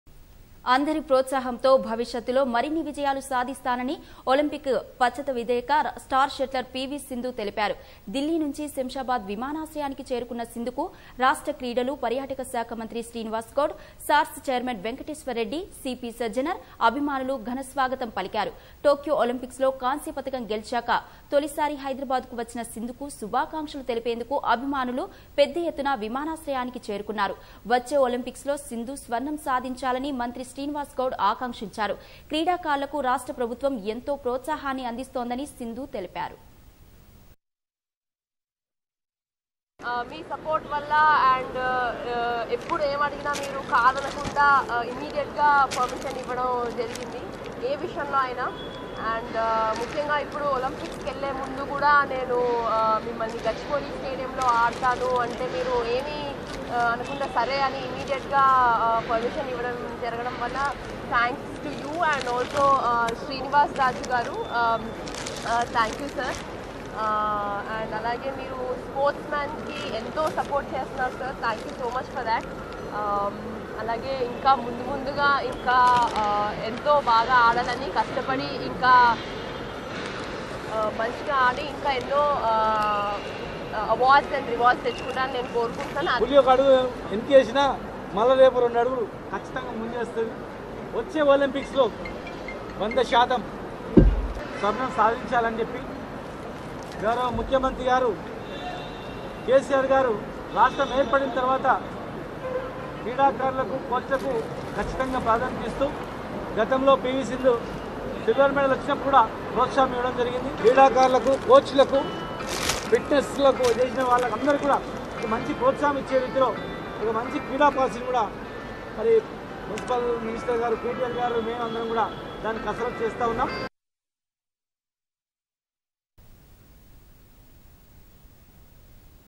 పీవీ సింధుకు శంషాబాద్‌లో ఘన స్వాగతం పలికిన అధికారులు | Grand Welcome to PV Sindhu in Hyderabad
అందరి ప్రోత్సాహంతో భవిష్యత్తులో మరిన్ని విజయాలు సాధిస్తానని ఒలింపిక్ పతక విజేత, స్టార్  షట్లర్  P.V.సింధు తెలిపారు. దిల్లీ నుంచి శంషాబాద్  విమానాశ్రయానికి చేరుకున్న సింధుకు రాష్ట్ర క్రీడలు, పర్యాటక శాఖ మంత్రి శ్రీనివాస్  గౌడ్ , సాట్స్  ఛైర్మన్  వెంకటేశ్వరరెడ్డి, సీపీ సజ్జనార్ , అభిమానులు ఘన స్వాగతం పలికారు. టోక్యో ఒలింపిక్స్ లో కాంస్య పతకం గెలిచాక తొలిసారి హైదరాబాద్ కు వచ్చిన సింధుకు శభాకాంక్షలు తెలిపేందుకు అభిమానులు పెద్ద ఎత్తున విమానాశ్రయానికి చేరుకున్నారు. వచ్చే ఒలింపిక్స్ లో సింధు స్వర్ణం సాధించాలని మంత్రి శ్రీనివాస్ గౌడ్ ఆకాంక్షించారు. క్రీడాకారులకు రాష్ట్ర ప్రభుత్వం ఎంతో ప్రోత్సాహాన్ని అందిస్తోందని సింధూ తెలిపారు